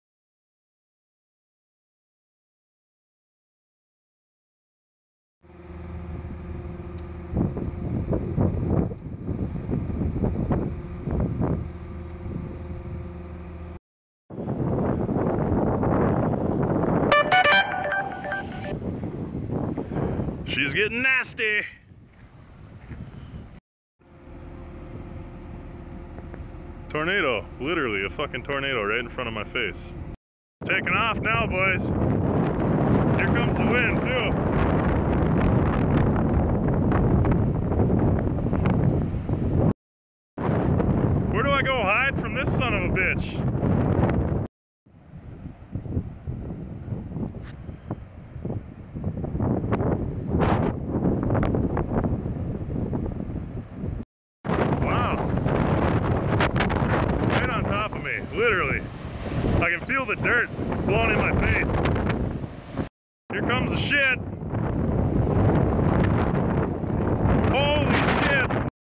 Snapchat footage shows fatal Otter Tail County tornado before it hits repair shop
Warning: Graphic content and language. Nate Erickson took these Snapchat videos of a tornado from the repair shop where Seth Nelson was killed.